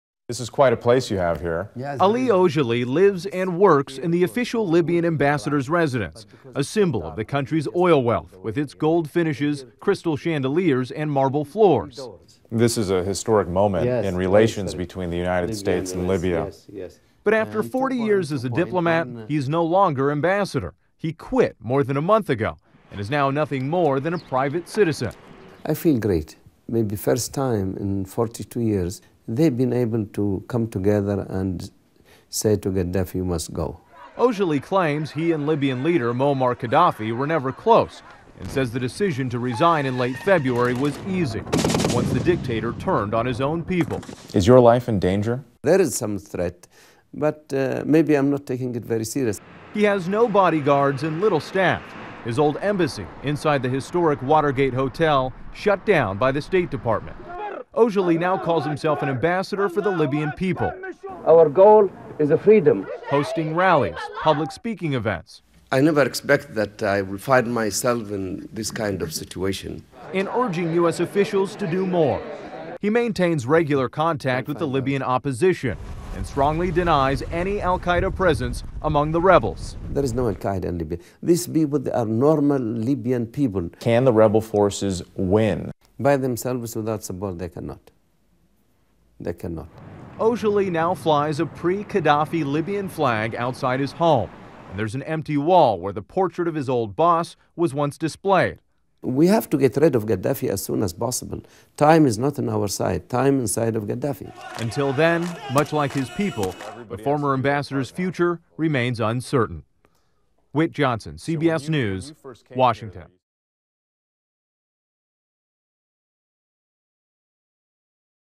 Former Libyan Ambassador speaks out
Former Libyan Ambassador to Washington Ali Aujali, who resigned his position over a month ago, is speaking out in support of the rebels. Whit Johnson reports on the uncertain future for Aujali and Libya.